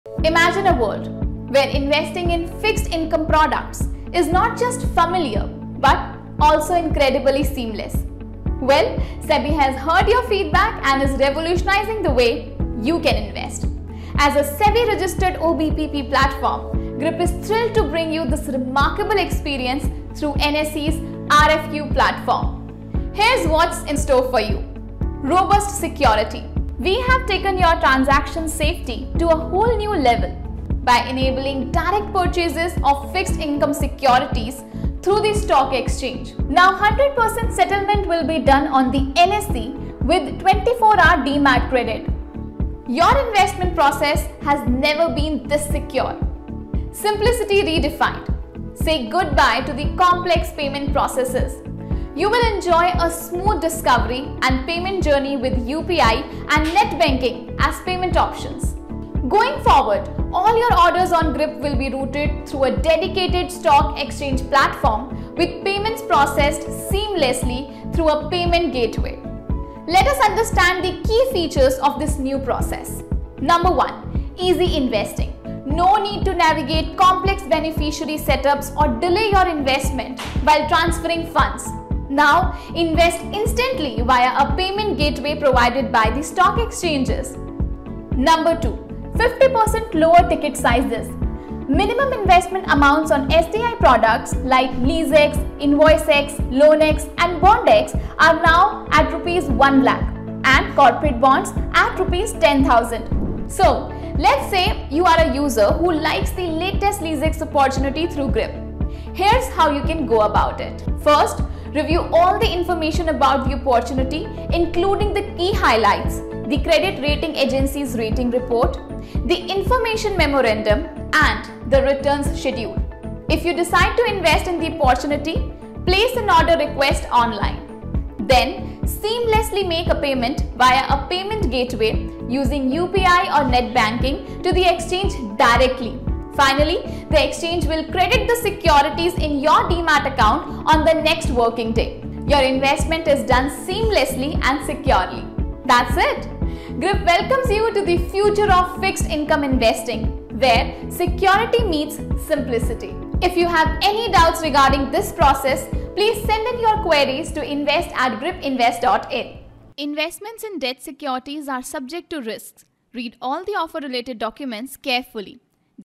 Grip Launches SEBI - Endorsed RFQ System | Grip Invest
Welcome to Grip 2.0 - "Less is More"

Embark on a more powerful investment journey with Grip Invest's latest upgrade, Grip 2.0, harnessing SEBI's RFQ mechanism! Driven by your feedback and the investment community's demand, we've evolved for a secure, convenient, and accessible fixed-income investment experience.

This integration brings less minimum investment, less settlement time, more ways to pay, and more secured transactions.

Investor Benefits:
1. Lower Minimum Investments: A remarkable 50%+ reduction in minimum investment for SDI options like LeaseX and LoanX
2. Diverse Payment Options: UPI and net banking are now available for seamless transactions
3. Swift Settlement: Securities in your Demat account within a single day (T+1 settlement)
4. Enhanced Security: All orders, payments, and settlements are handled via NSE for added security